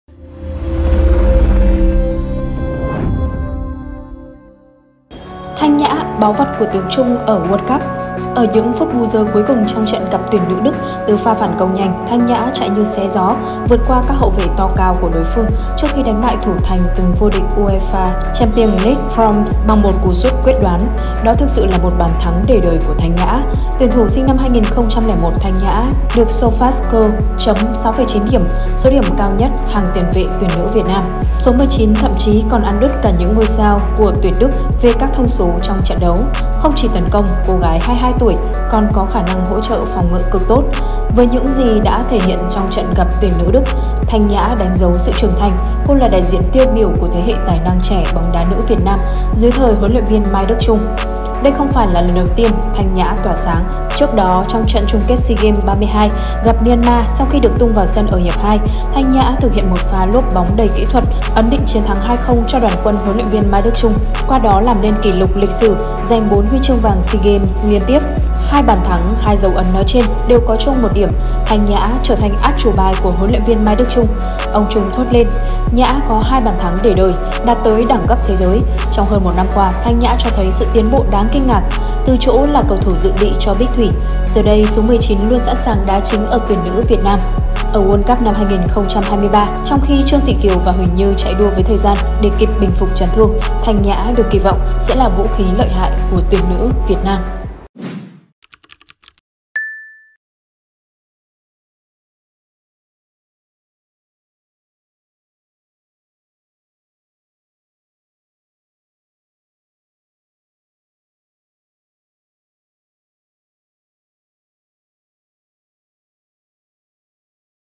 Thanh Nhã: "Báu vật" của HLV Mai Đức Chung ở World Cup 2023 | Báo VietNamNet
Với 2 bàn thắng để đời tại SEA Games 32 và mới nhất là trong trận giao hữu giữa Việt Nam và đội tuyển Đức, "Cơn lốc đường biên" Thanh Nhã hứa hẹn là nhân tố có thể tạo nên khác biệt cho tuyển nữ Việt Nam ở World Cup 2023.